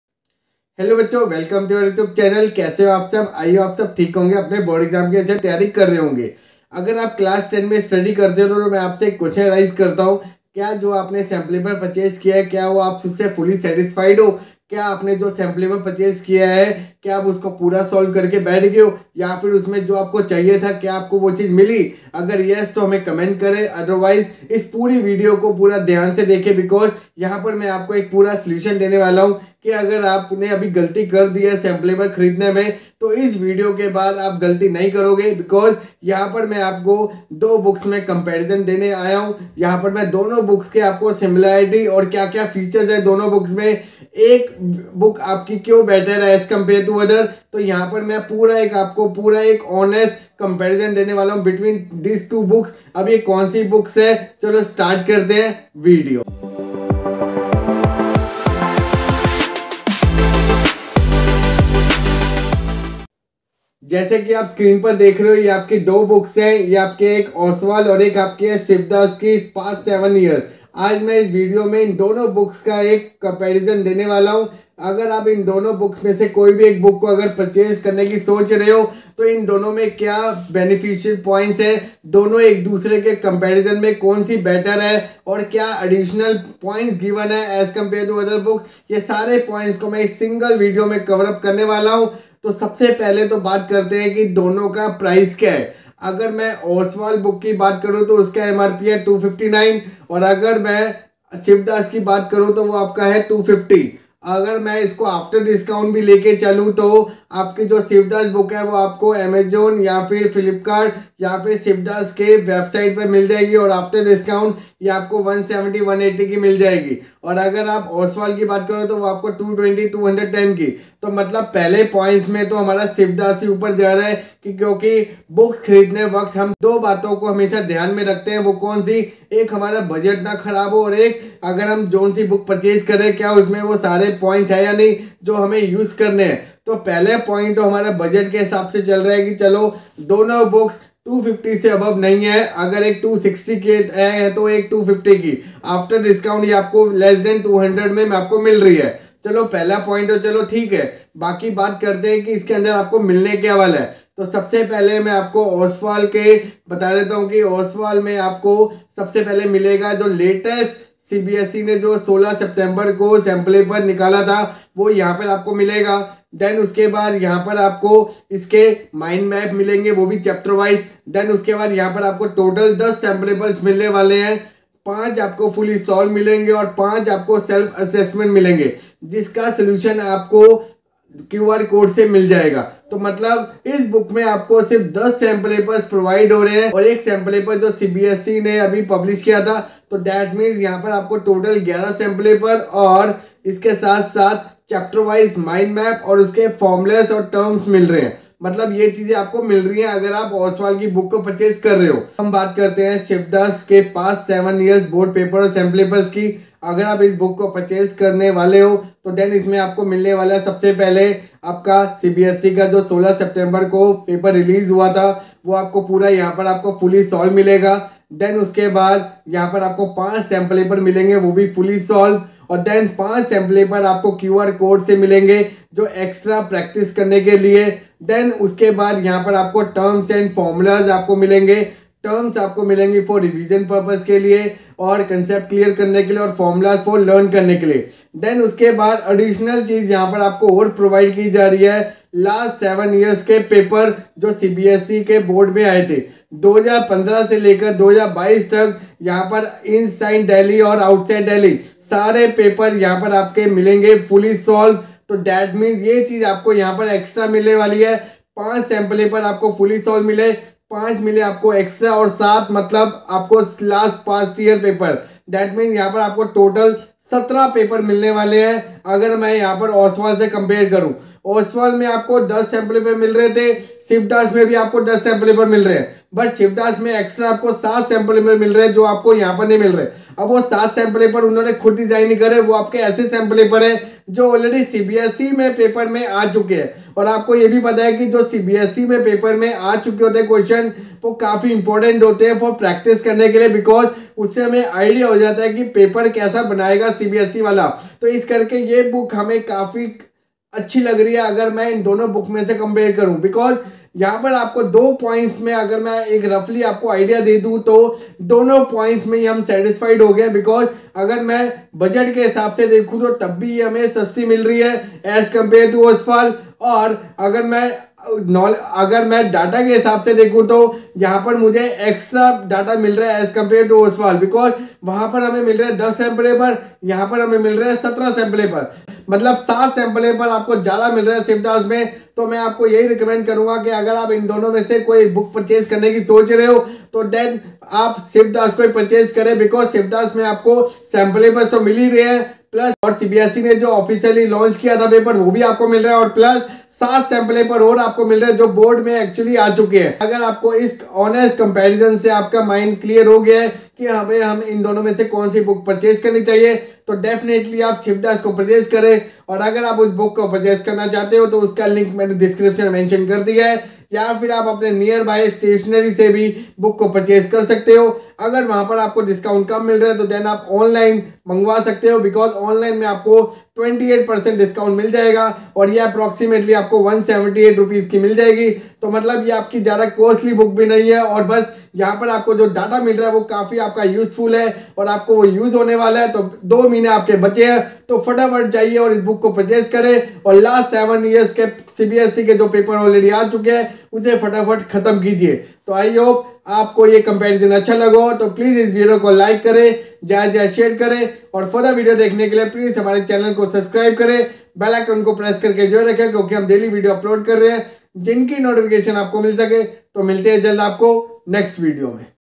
Best Sample Paper For Class 10 CBSE For 2023 Exam | Shivdas or Oswaal
*IMPORTANT*
ShivDas Past 7 years Board Papers + Sample Papers Book includes :

 • Latest CBSE Sample Paper ( Fully Solved )
• 5 Sample Papers ( Fully solved )
• 5 More Sample Papers ( Download from QR Code )
• 14 Past Years { ( 2015 to 2022 ) Inside & Outside Delhi } CBSE Board Papers

*IMPORTANT* - You can get all books on Amazon or your nearby stationery. I have also attached the Purchase link for sample papers Books.